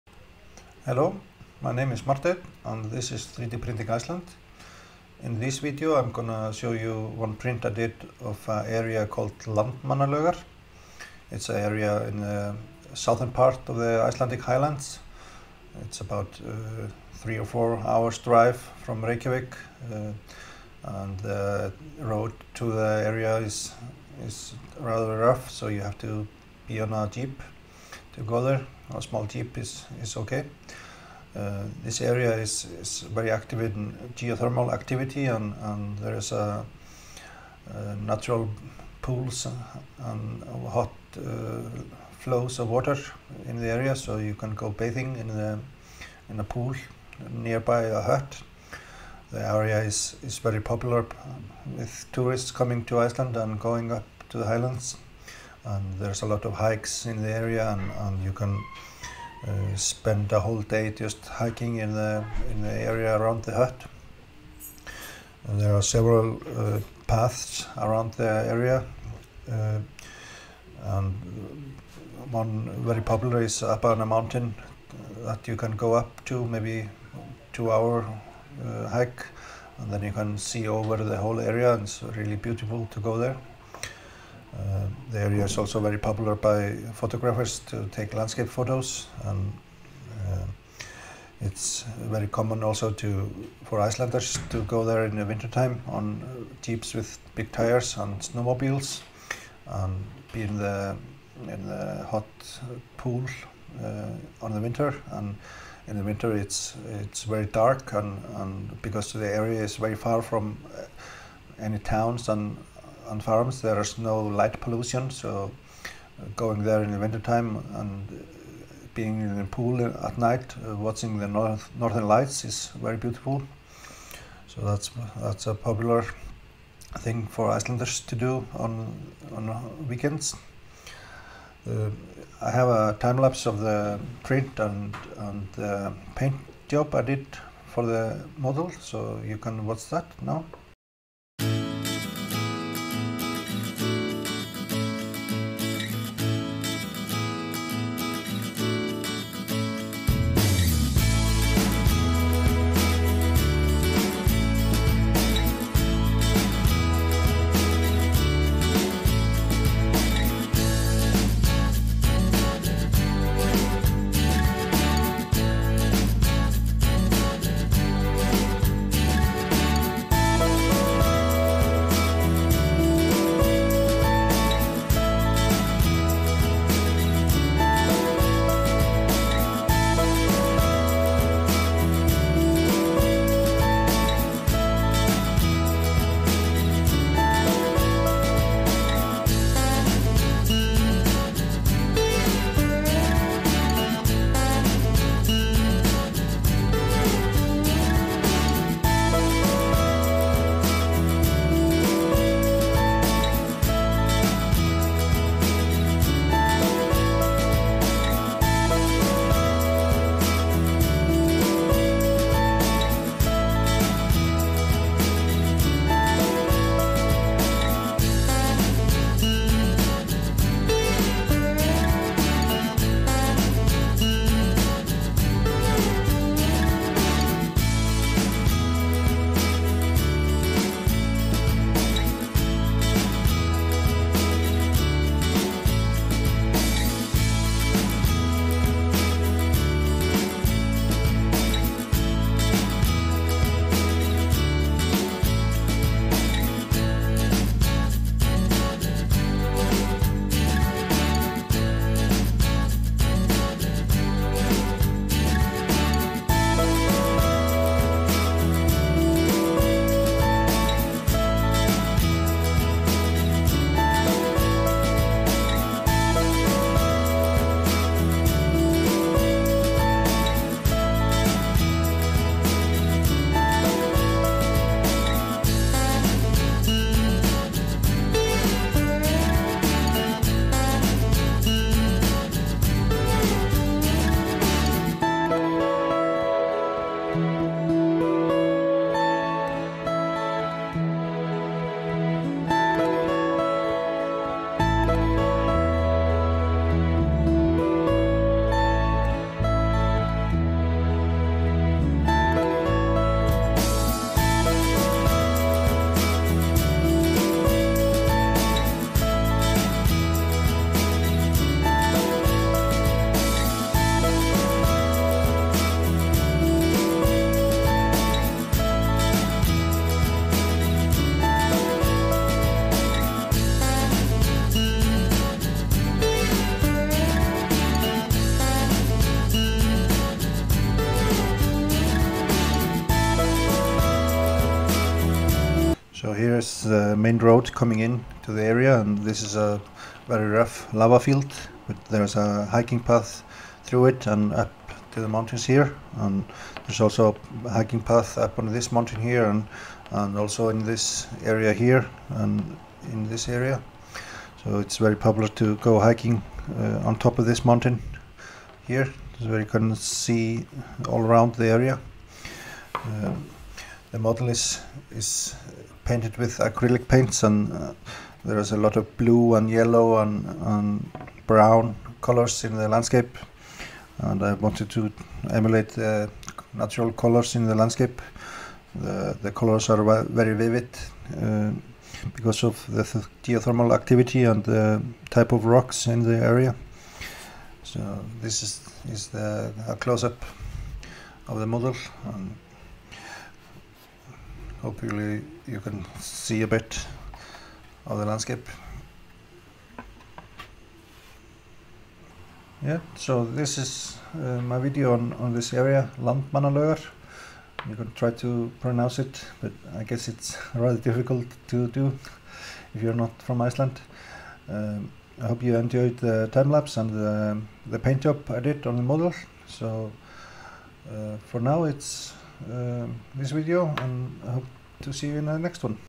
Landmannalaugar - 3D Printed and Painted
In this video I print out and paint a area in the southern part of the Icelandic highlands. The area is called Landmannalaugar and it's very popular tourists area and also for Icelanders to go there as well.